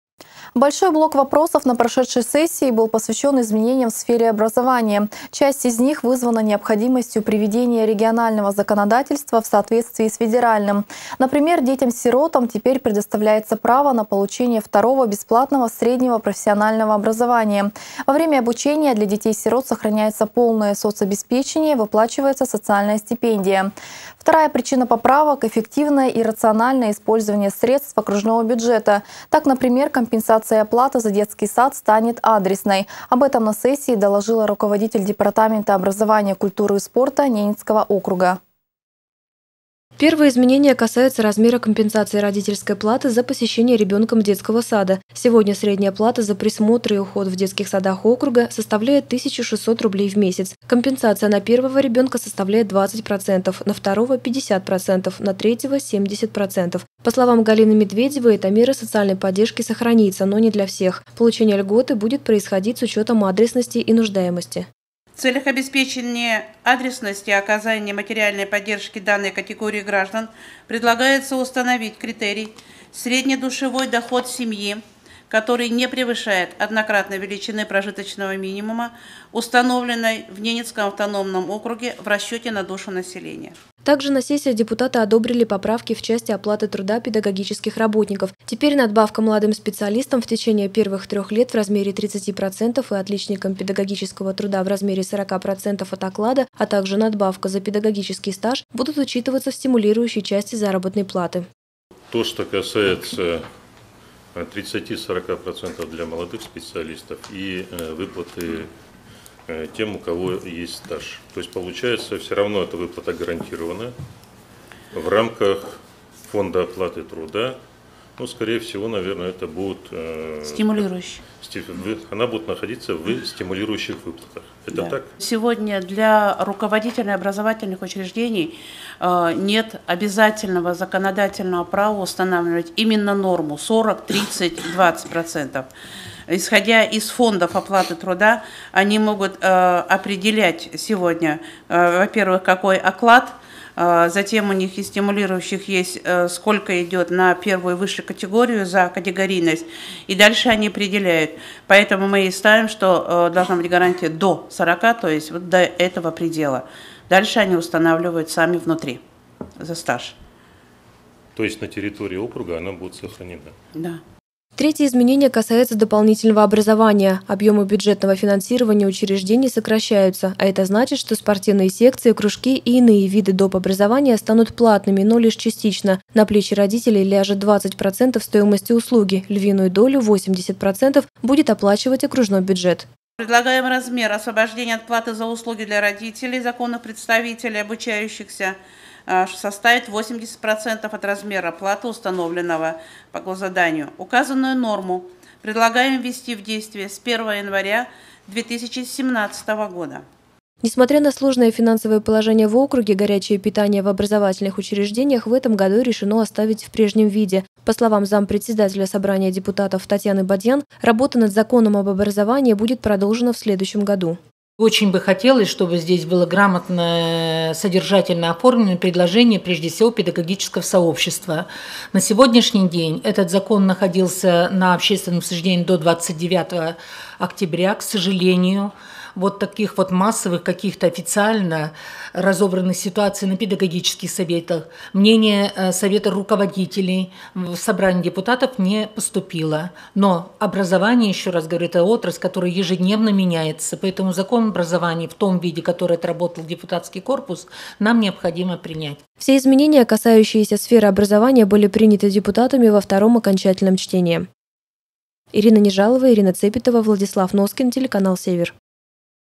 Поправки в окружной «Закон об образовании» приняты в окончательном чтении
Образование детей-сирот, горячее питание в школах, плата за кружки и надбавки молодым специалистам. Большой блок вопросов на очередной сессии Собрания депутатов НАО был посвящен изменениям в сфере образования. Часть из них связана с необходимостью приведения регионального законодательства в соответствие с федеральным. Например, детям-сиротам теперь предоставляется право на получение второго бесплатного среднего профессионального образования. Во время обучения для детей-сирот сохраняется полное соцобеспечение, выплачивается социальная стипендия. Вторая причина поправок — эффективное и рациональное использование средств окружного бюджета. Так, например, компенсация оплаты за детский сад станет адресной. Об этом доложила руководитель Департамента образования, культуры и спорта Ненецкого округа.

Первое изменение касается размера компенсации родительской платы за посещение ребенком детского сада. Сегодня средняя плата за присмотр и уход в детских садах округа составляет 1 600 рублей в месяц. Компенсация на первого ребенка составляет 20%, на второго — 50%, на третьего — 70%. По словам Галины Медведевой эта мера социальной поддержки сохранится, но не для всех. Получение льготы будет происходить с учетом адресности и нуждаемости — среднедушевой доход семьи не должен будет превышать однократной величины прожиточного минимума, установленной в Ненецком автономном округе в расчете на душу населения.

Также на сессии депутаты одобрили поправки в части оплаты труда педагогических работников. Теперь надбавка молодым специалистам в течение первых трех лет в размере 30% и отличникам педагогического труда в размере 40% от оклада, а также надбавка за педагогический стаж будут учитываться в стимулирующей части заработной платы.

Третье изменение касается дополнительного образования. Объемы бюджетного финансирования учреждений сокращаются, а это значит, что спортивные секции, кружки и иные виды дополнительного образования станут платными, но лишь частично. На плечи родителей ляжет 20% стоимости услуги, львиную долю — 80% — будет оплачивать окружной бюджет. Указанная норма начнет действовать с 1 января 2017 года.

Несмотря на сложное финансовое положение в округе, горячее питание в образовательных учреждениях в этом году решено оставить в прежнем виде. По словам заместителя председателя Собрания депутатов Татьяны Бадьян, работа над законом об образовании будет продолжена в следующем году.

Все изменения, касающиеся сферы образования, были приняты депутатами во втором окончательном чтении.